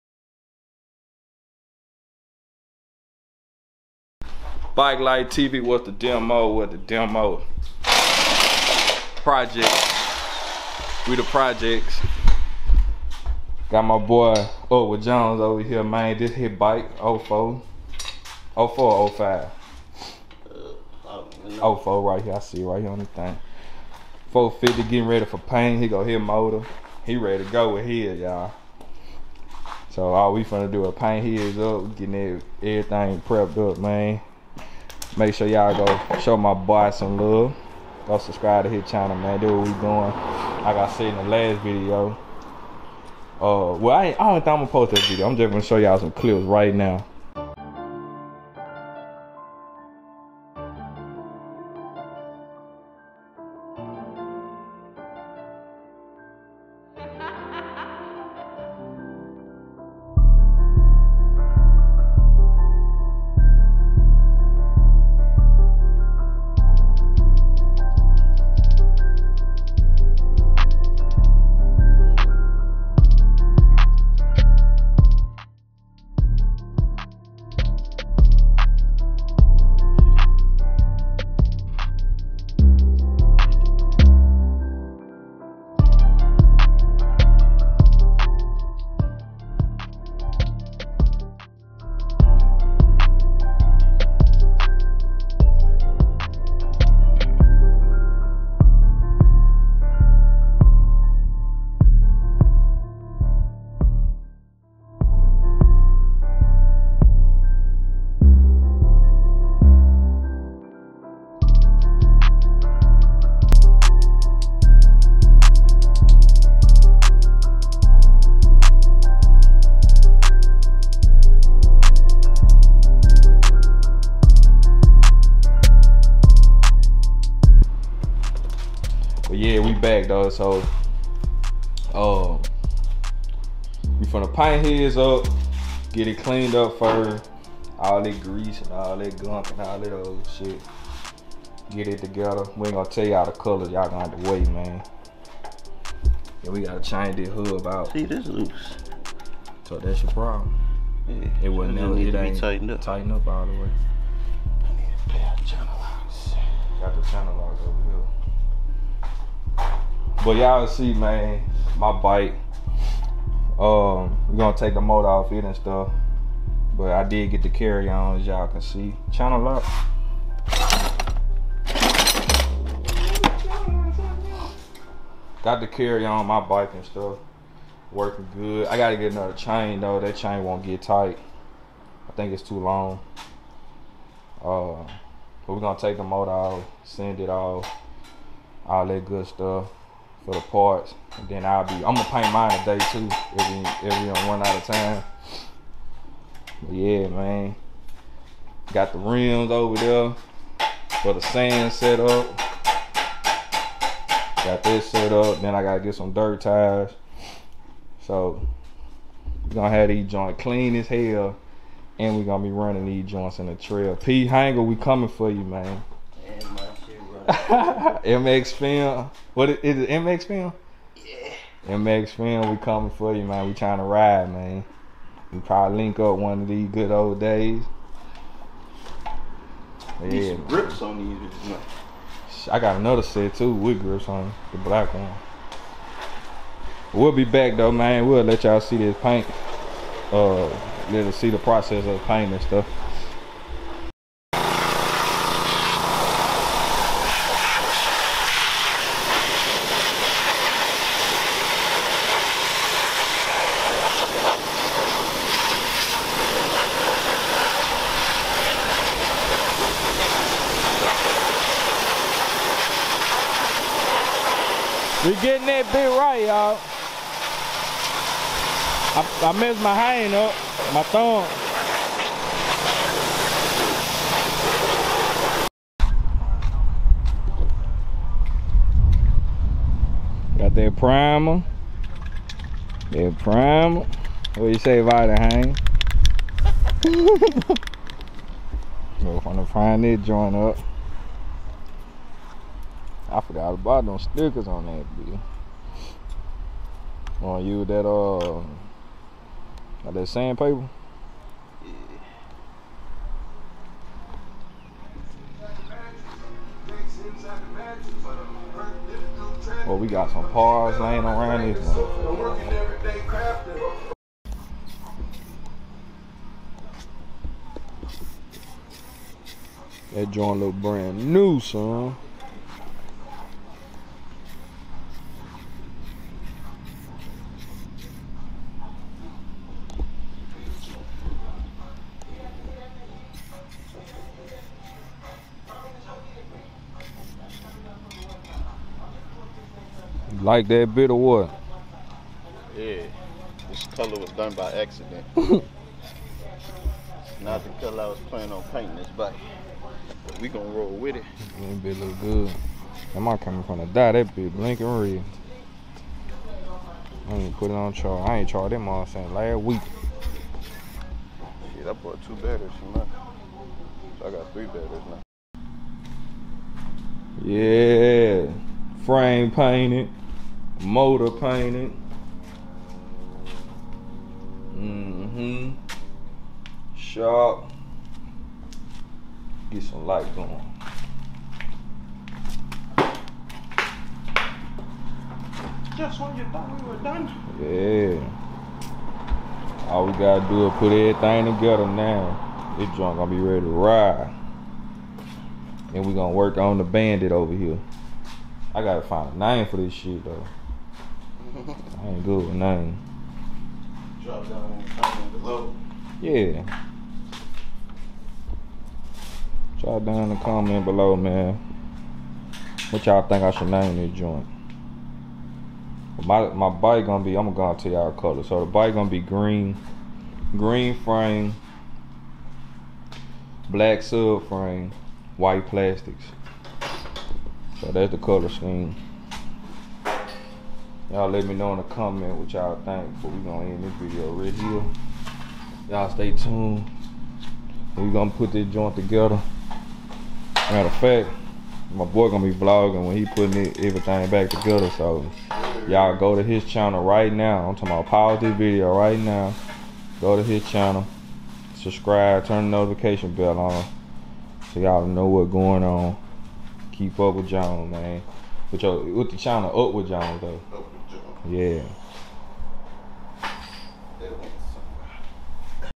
2004 Trx450r Gets Full Rebuild | @upwithjones2298 | 2021
Yes sir my boy came over with his bike and we stripped it down to the frame and painted the frame, motor, a arms, swing arm, etc. We getting ready for the trail rides and sand dunes. We will be drag racing them also. Stay Tune for my build 2006 trx450er.